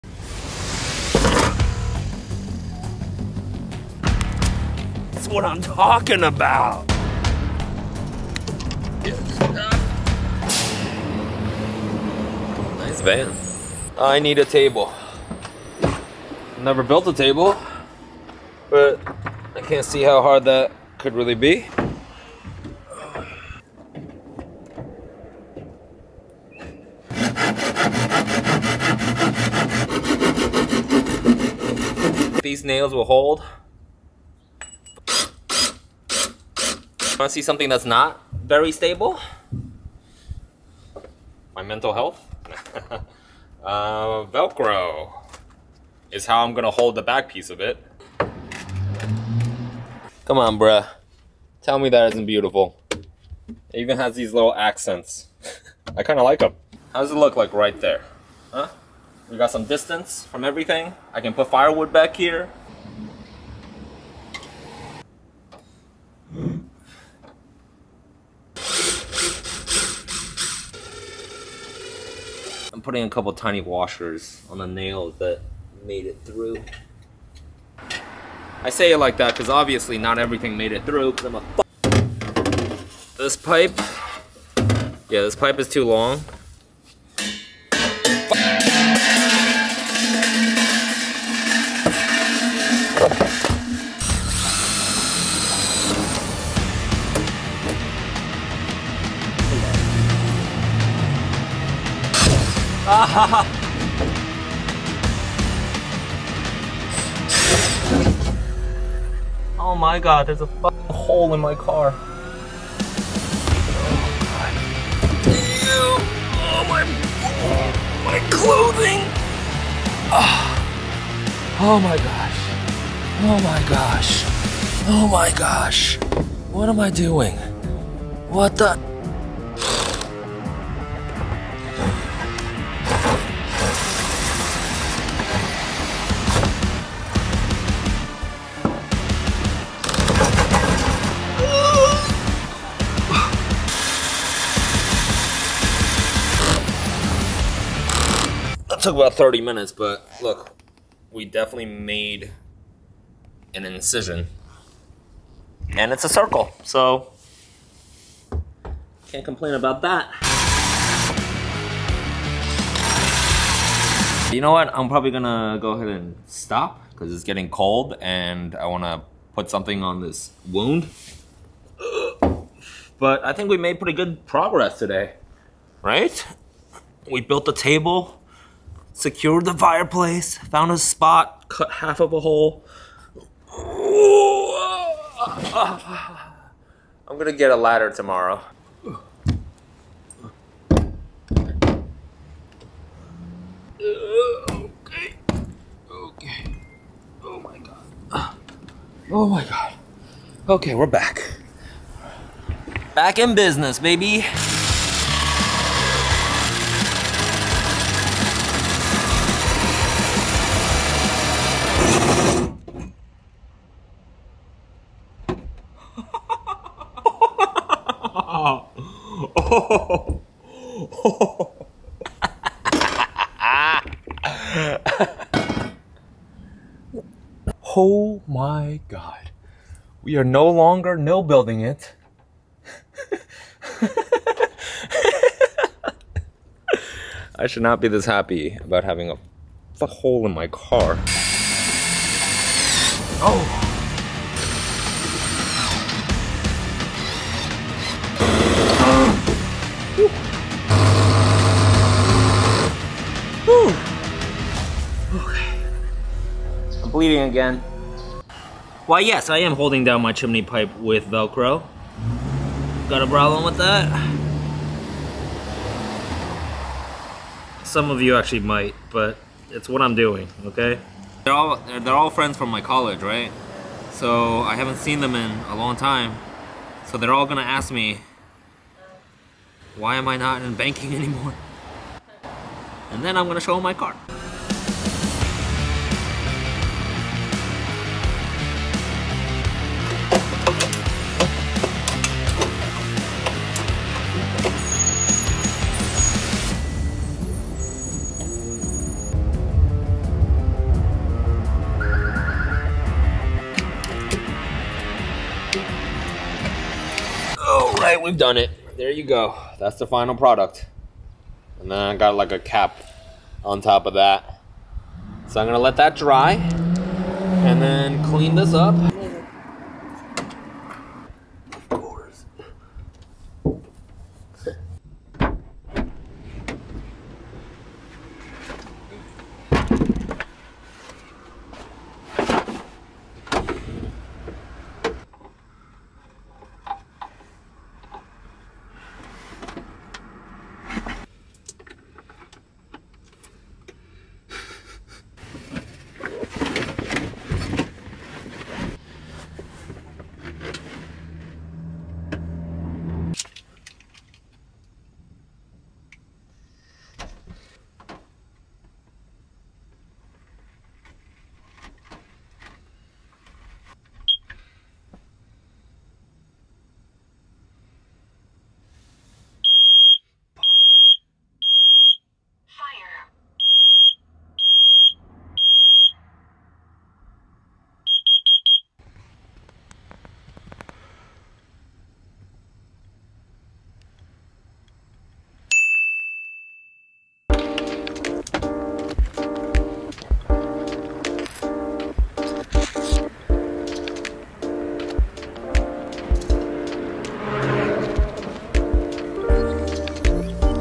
Wood Burning Stove Install to Heat my Car | Solo Winter Vanlife
Hey friends,

Here is a quick video of my solo installation of my Mini Cubic Grizzly Wood Stove. Out of the few terribly boring heating options for cars I have chosen wood (over gas, diesel, electric, propane).
Bruh it's just hella romantic and as a camping enthusiastic I love burning, wood, being off grind, the smell, the danger, etc. everything about it just sounds fun. And if it happens to keep me warm this winter, all the better.

Winter is the superior camping season, NO BUGS.

Thank you and kind regards,
Jack + Apps